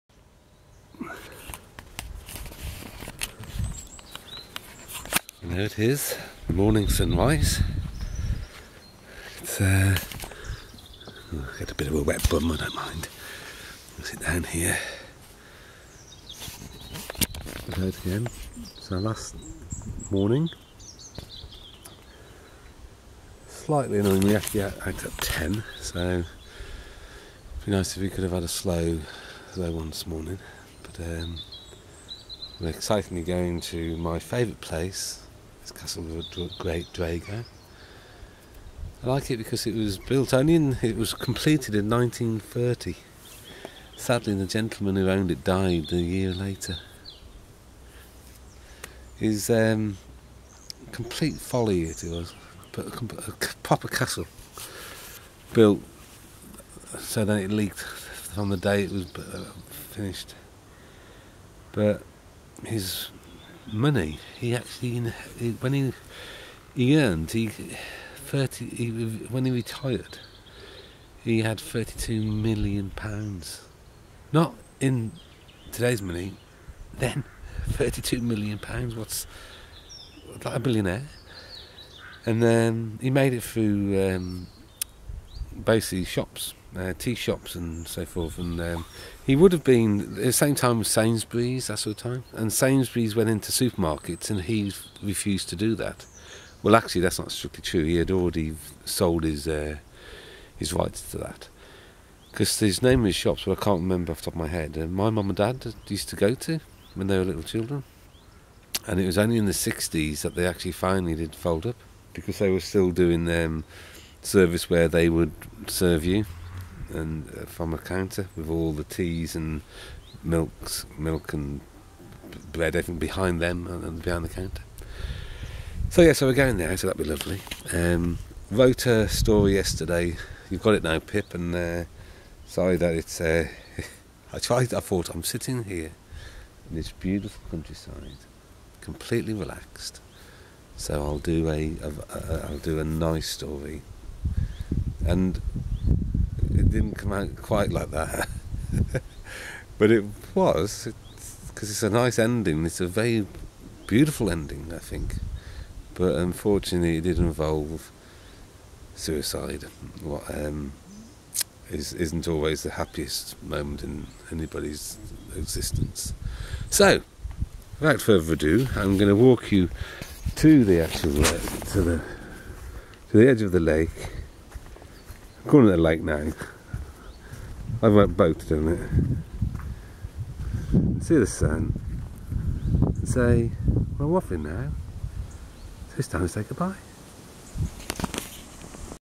David Banfords daily woffle thoughts 27/9/20
Sun rise in the shepherd's Hut, Lovely for our last morning here back to the red dot in a blue cloud tonight.